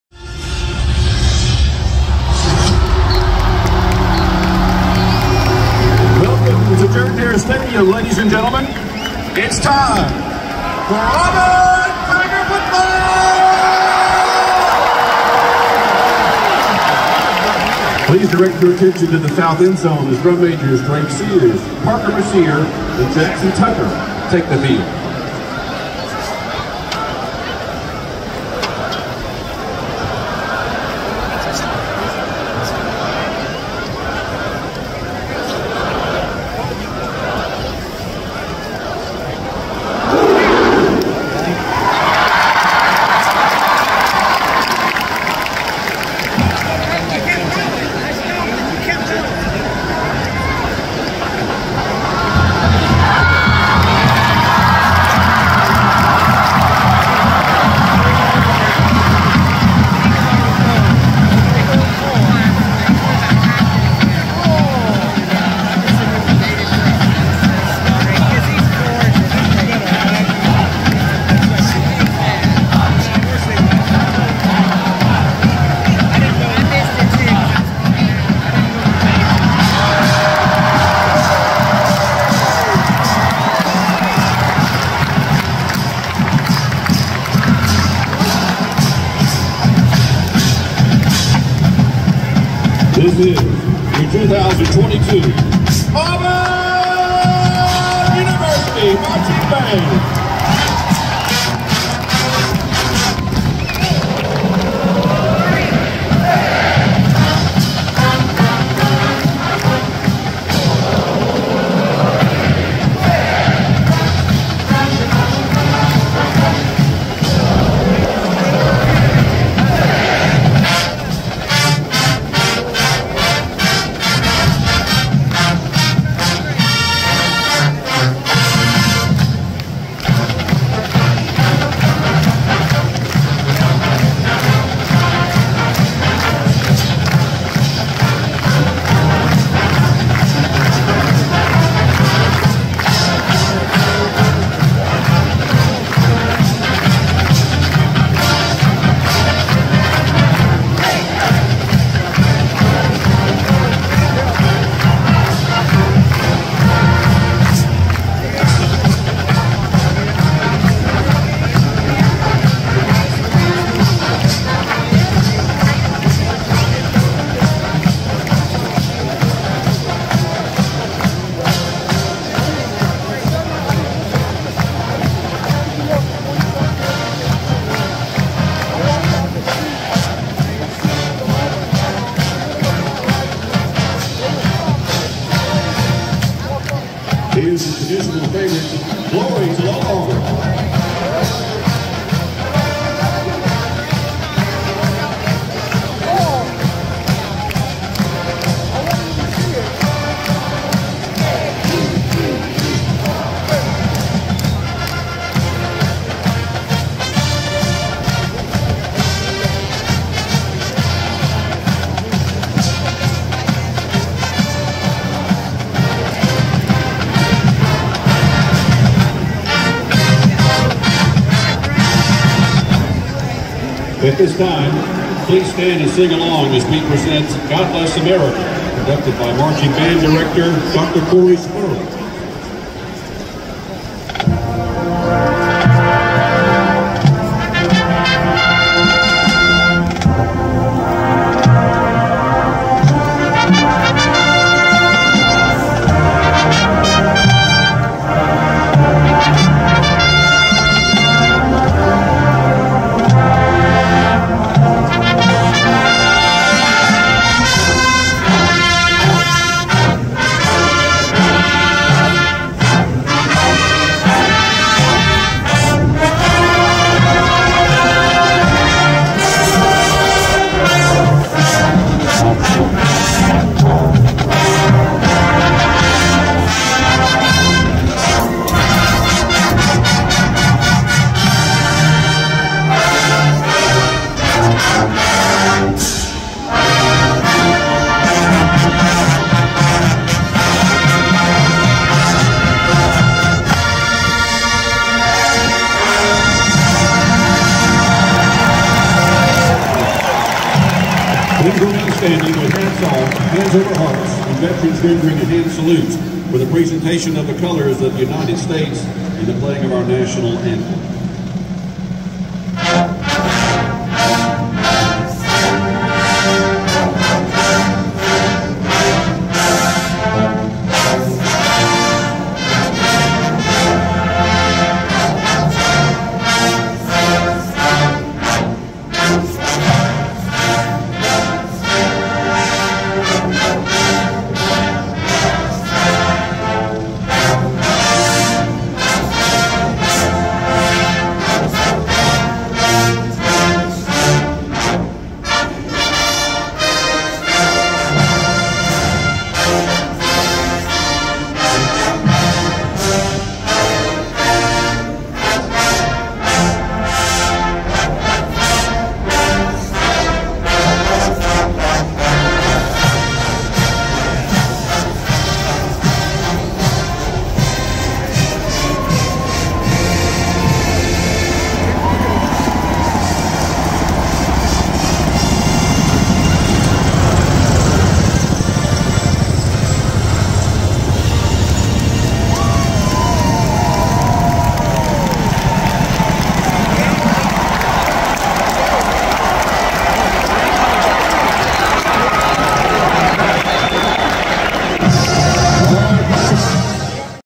2022 Auburn University Marching Band - Game 8 - Pre-Game vs Arkansas Razorbacks
2022 Auburn University Marching Band - Game 8 - Pre-Game vs Arkansas Razorbacks